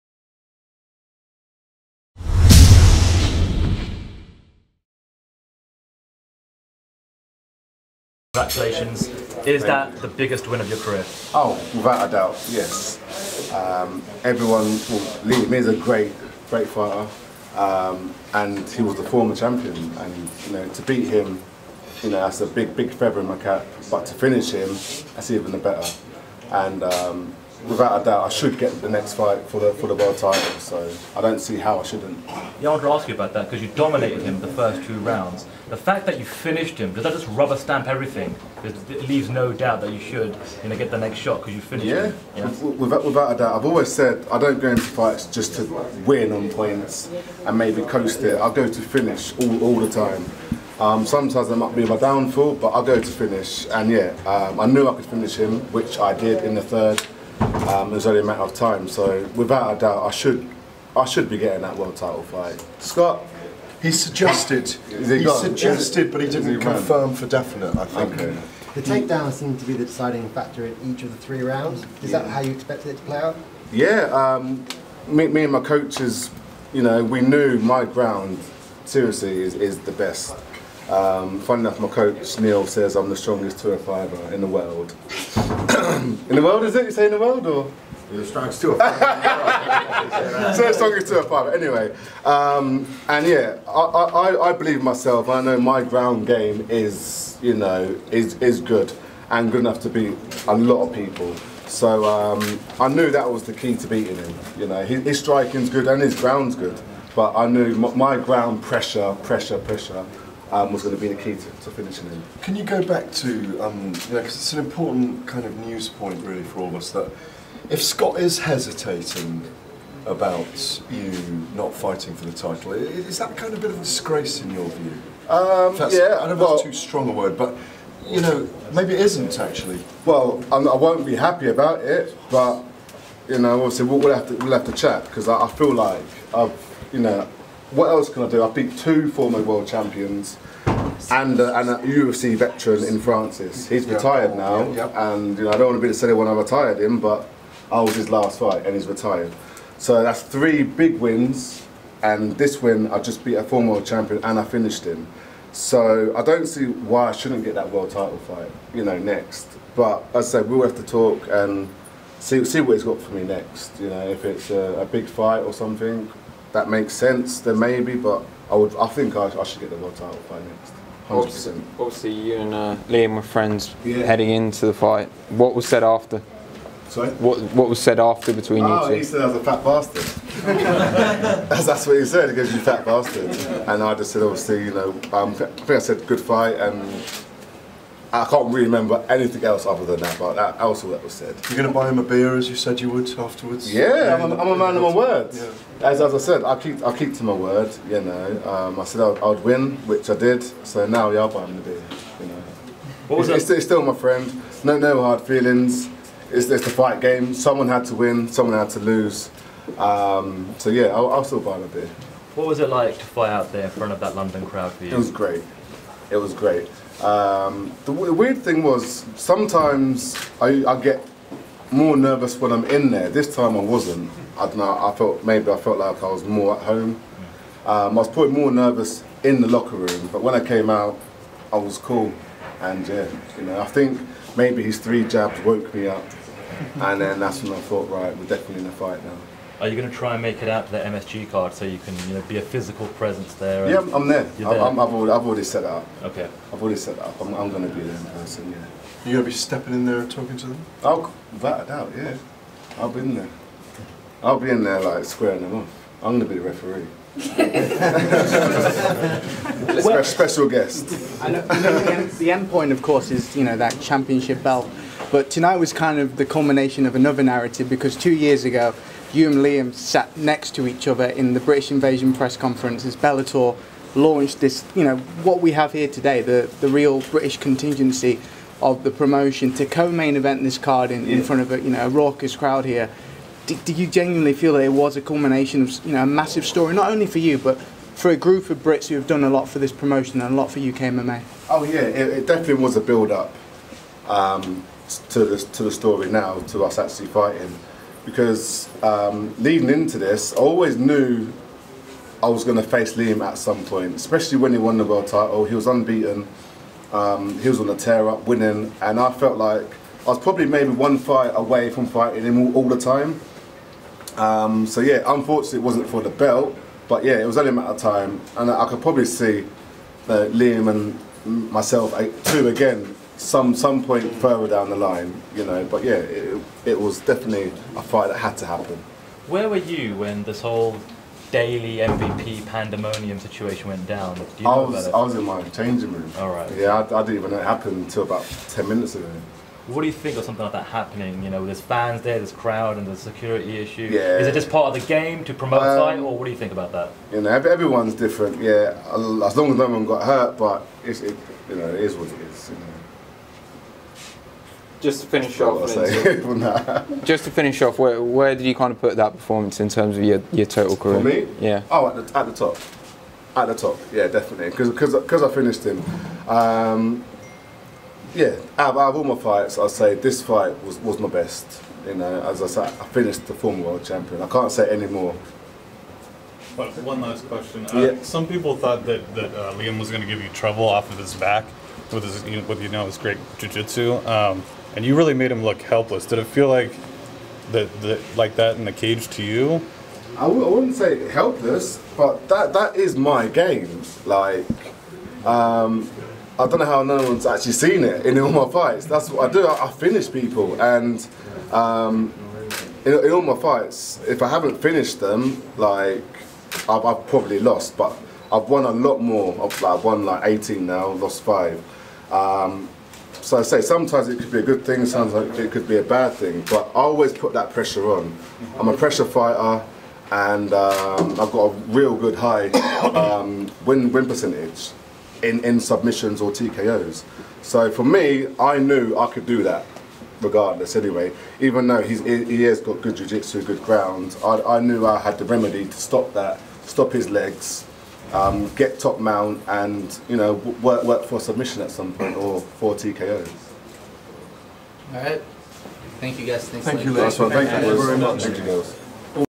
Linton Vassell states that he is ready for Phil Davis and Ryan Bader next
Linton Vassell Bellator 179 Post Fight Media Scrum

Linton Vassell talks submission win over Liam McGeary at Bellator 179 and states that he is ready for Phil Davis and Ryan Bader next

Bellator 179: Daley vs. MacDonald

Bellator Media Scrum